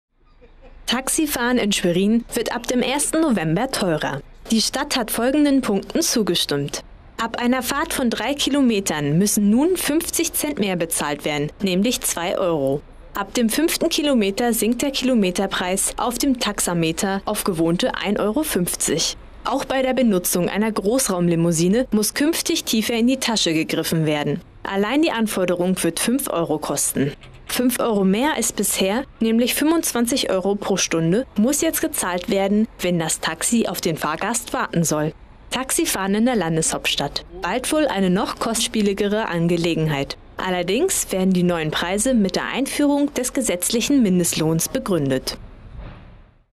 Fahrpreise steigen
Taxifahren in Schwerin soll teurer werden.